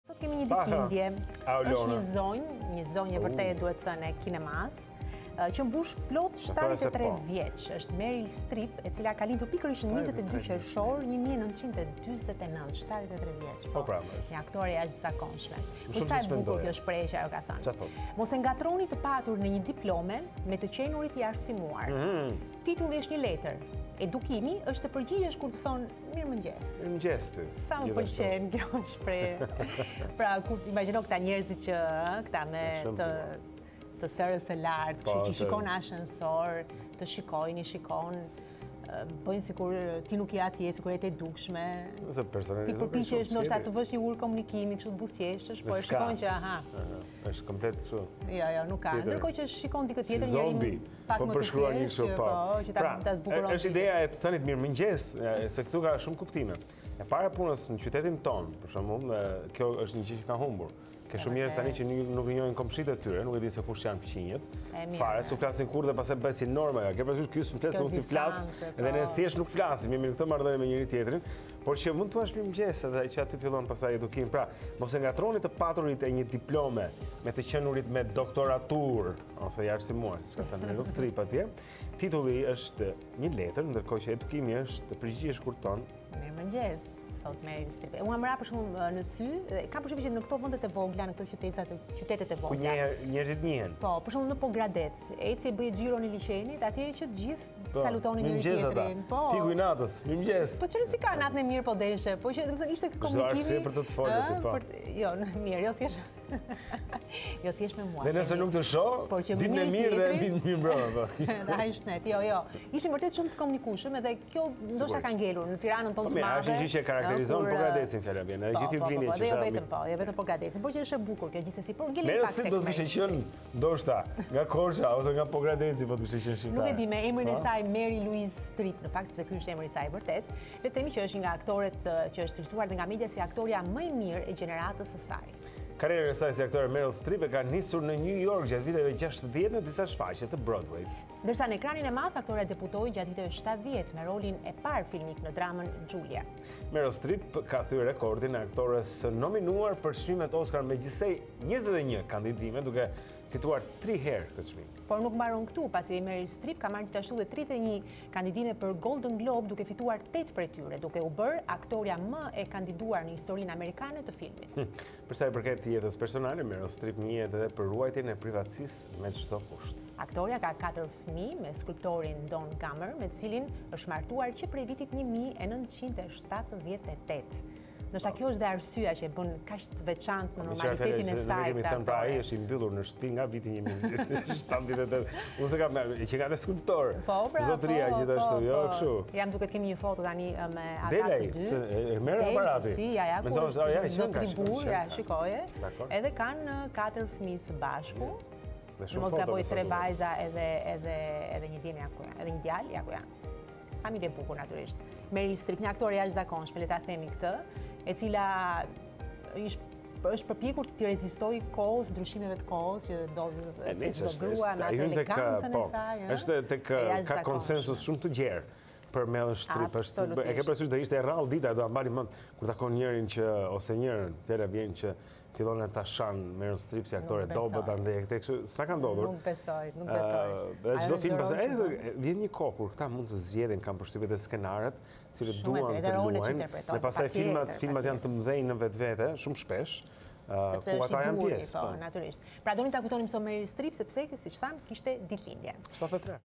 Meryl Streep feston 73-vjetorin, njihet si "aktorja më e mirë e brezit të saj
Mary Louise Streep është cilësuar nga media si "aktorja më e mirë e gjeneratës së saj". Karrierën e saj si aktore, Streep e ka filluar në New York gjatë viteve ‘60, në disa shfaqje të Broadway-it. Ndërsa në ekranin e madh, aktorja debutoi gjatë viteve ‘70 me rolin e parë filmik në dramën e “Julia”.